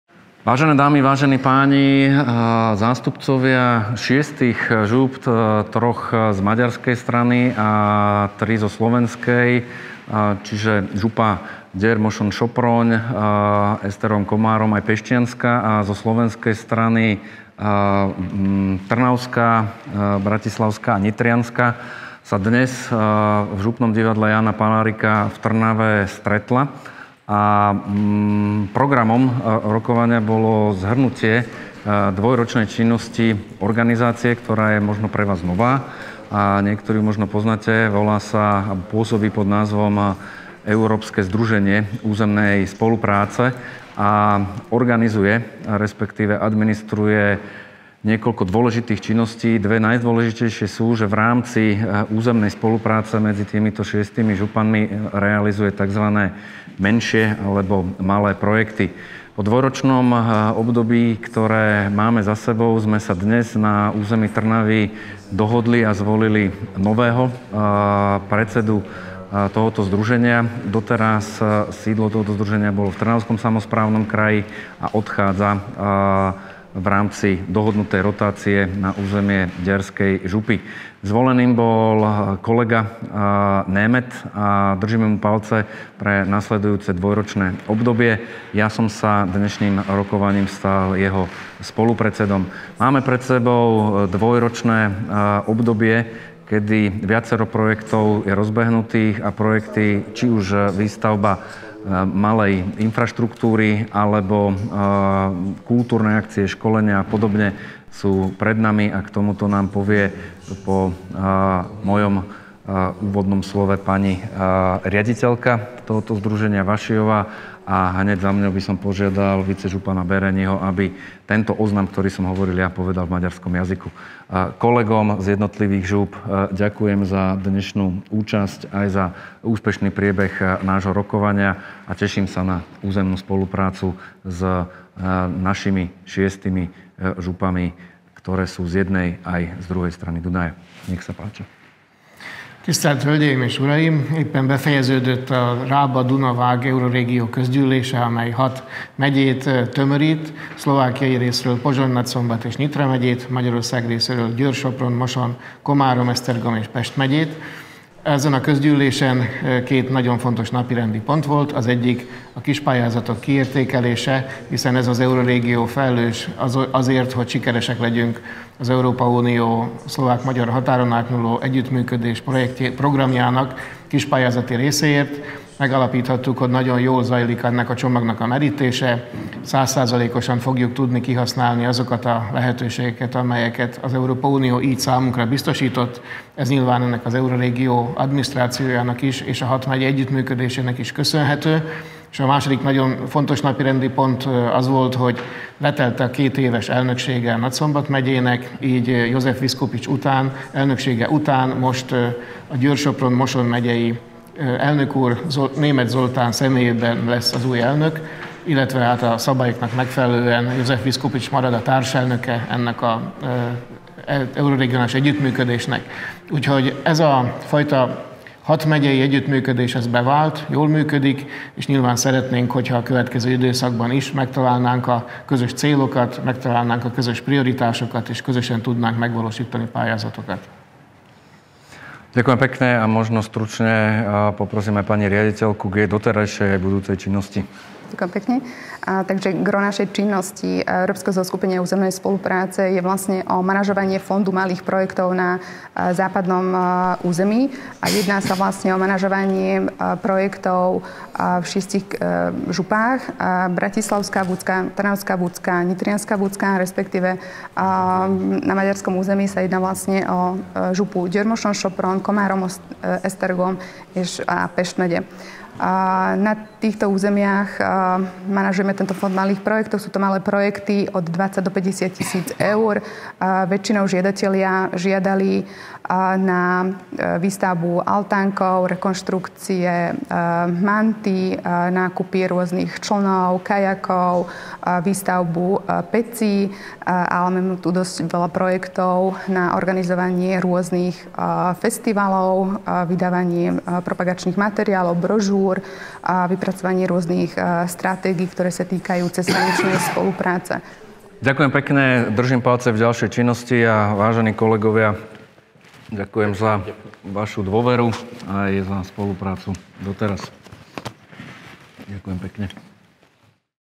Vďaka spolupráci susedných žúp môžu byť podporené verejnoprospešné projekty
Rekonštrukcie turistických atrakcií, výstavba komunitných zariadení, podpora festivalov, táborov pre deti aj výskumné projekty.
Finančná pomoc pre tieto projekty je možná vďaka spolupráci 3 slovenských a 3 maďarských žúp zoskupených v Zhromaždení Európskeho zoskupenia územnej spolupráce Rába-Dunaj-Váh (EZÚS R-D-V).